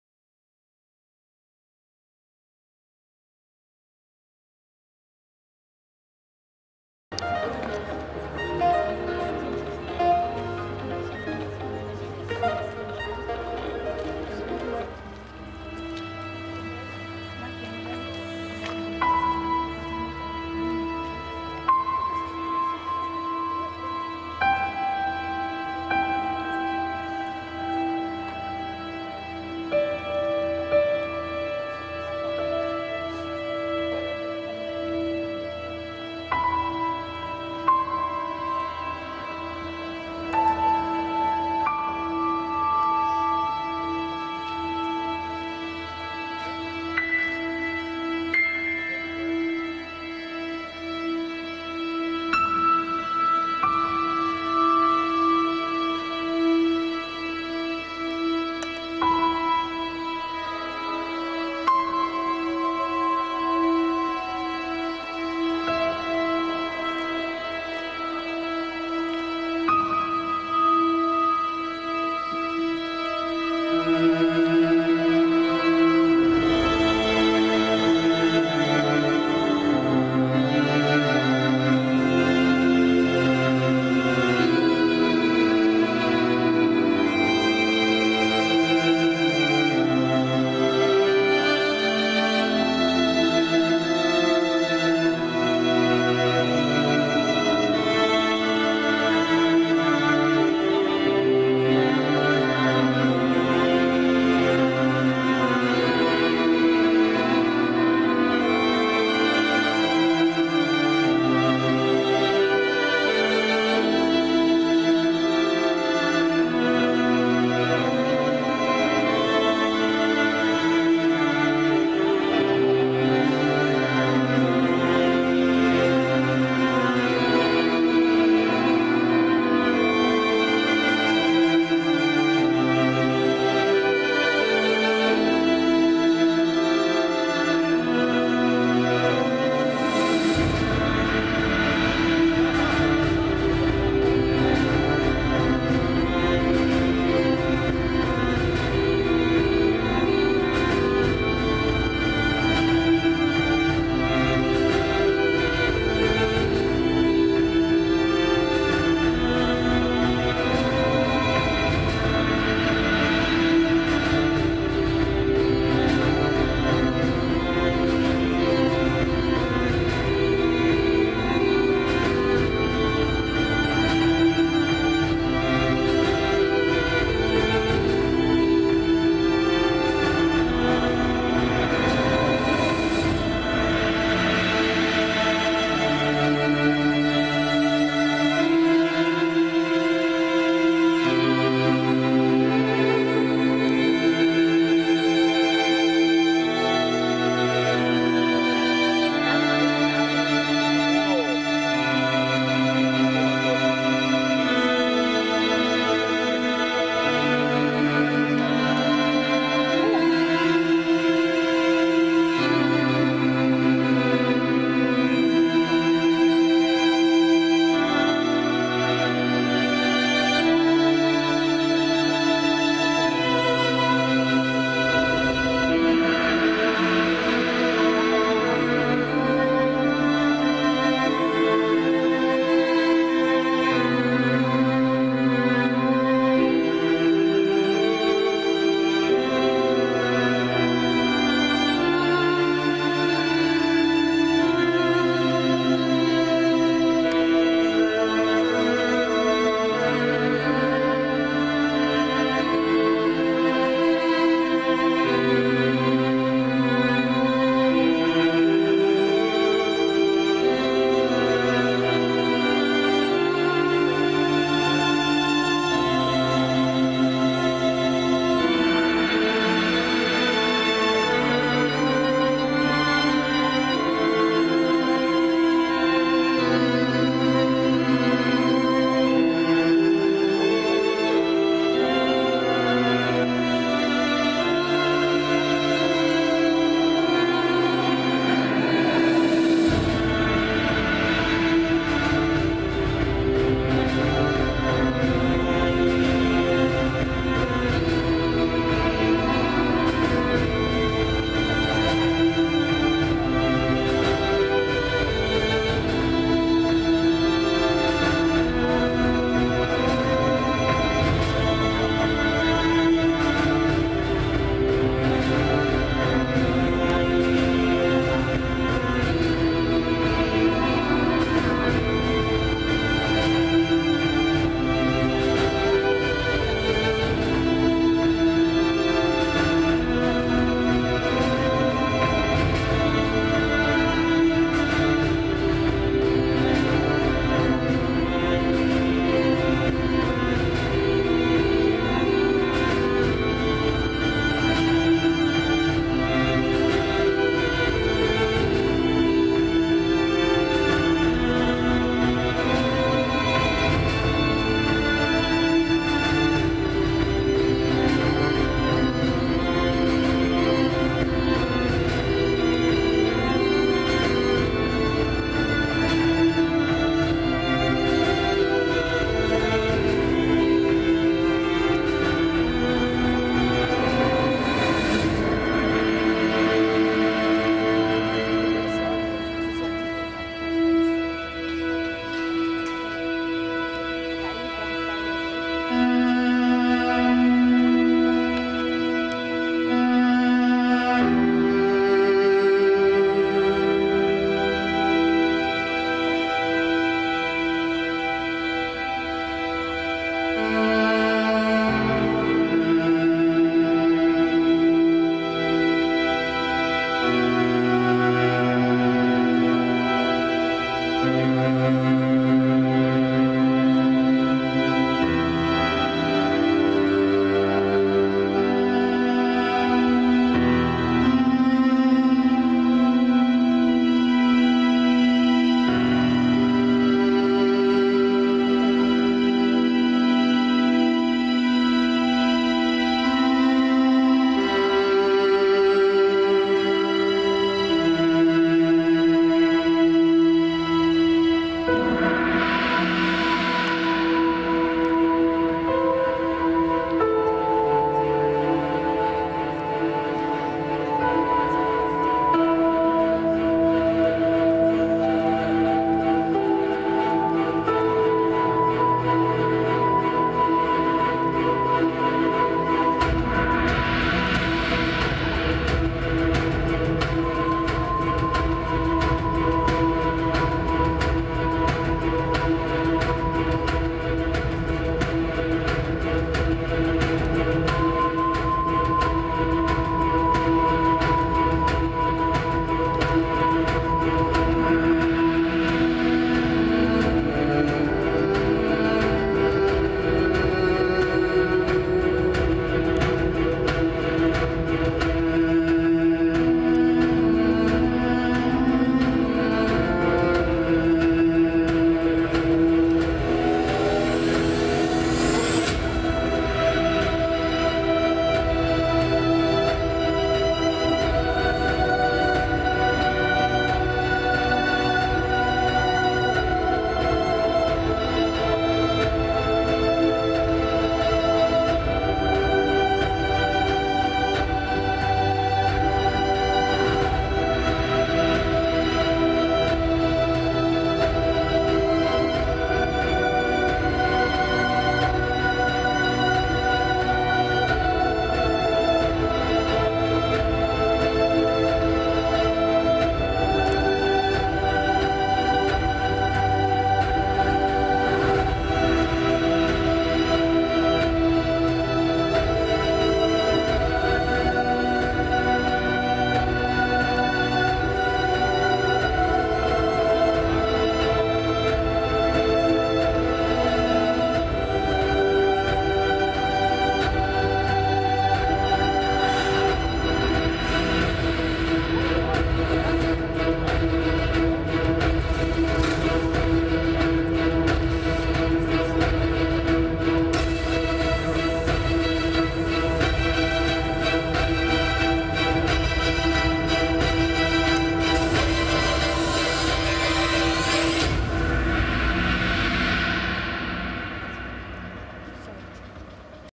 Genius Loci Weimar 2018 - Haus der Frau von Stein 1080p50
HAUS DER FRAU VON STEIN (1080p50/DD5.1)

GEWINNERBEITRAG 2018
"Musae"

Für ihre Produktion 'Musae' haben MultiScalar auf exemplarische Weise die Historie des Ortes mit seiner architektonischen Struktur, die an eine Theaterbühne erinnert, in Verbindung gebracht. Mit den Mitteln moderner medialer Gestaltung, mit Animation, Simulation und Abstraktion wird der Dialog und die Dialektik Muse versus Genie inszeniert. Besondere Anerkennung findet neben der ausführlichen Stoffentwicklung das Audiodesign, das das Duett der Protagonisten in seiner Entwicklung von einer anfänglichen Disharmonie hin zu einem triumphalen Finale gekonnt betont.